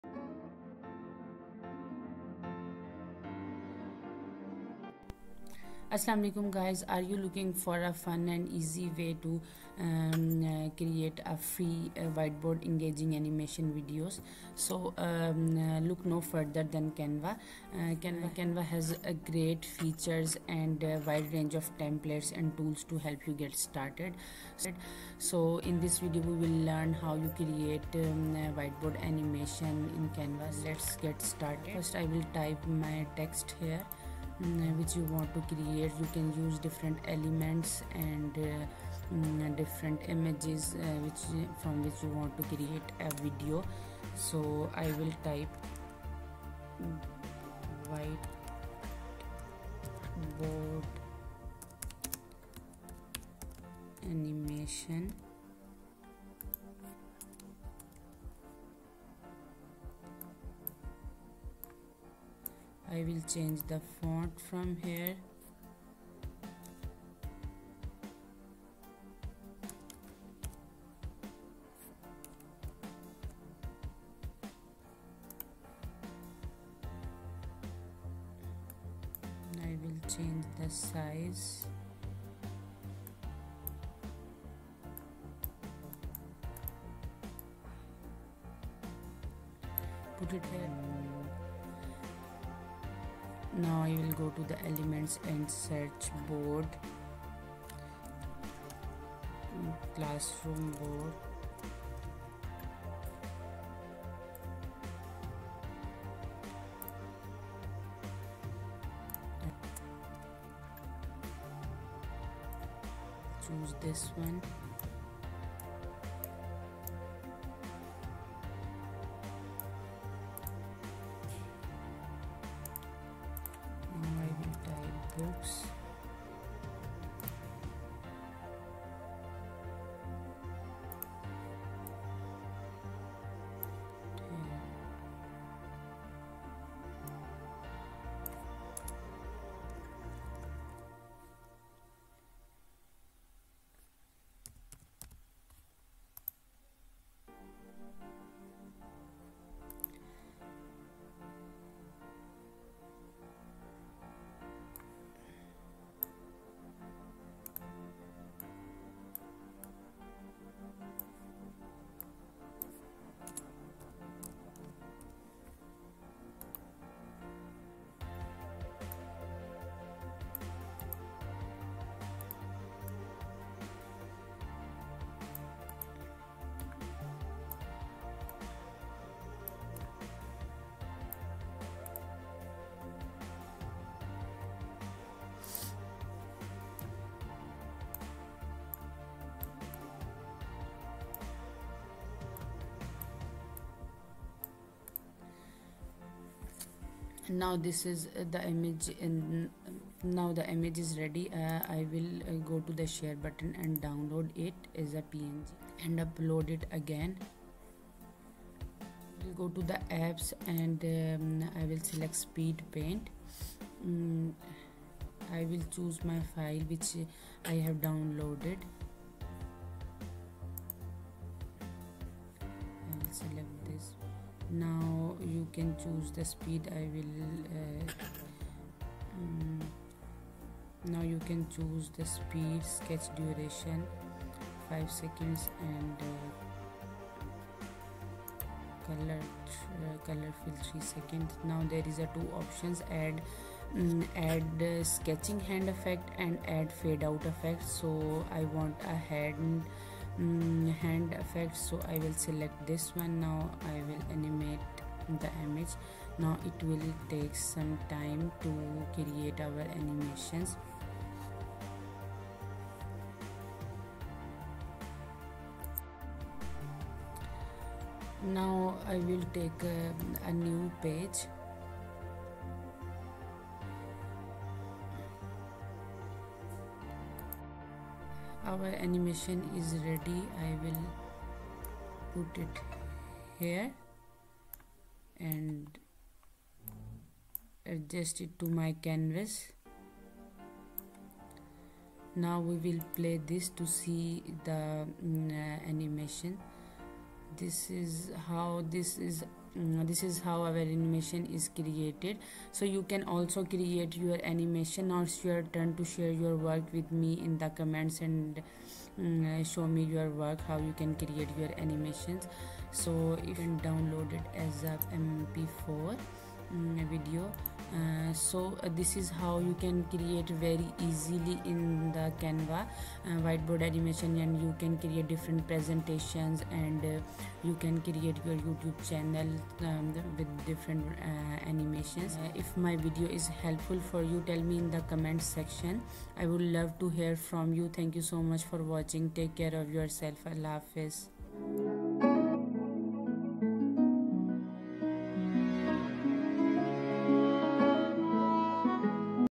Make Money Online 2024 with Whiteboard Animation Free BY Using AI Canva
Master the art of whiteboard animation with this clear and concise Canva tutorial. Learn every step, from choosing templates to adding stunning effects, and transform your ideas into eye-catching videos, even with zero animation experience.